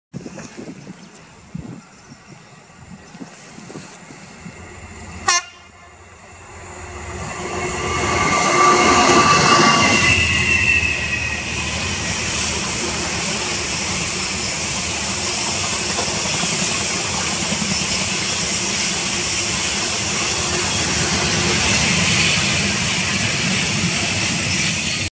66090
66090 passing Thankerton with 6M51 the 06.20 Millerhill to Carlisle New yard on Friday 26 April 2019